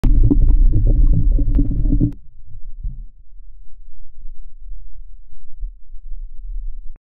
Small-Jaw Swipes Swimbait
A unique underwater perspective of a big smallmouth bass hammering a Berkley Champ Swimmer rigged on a Trokar Swim Blade. I’m still in shock that we actually got this shot.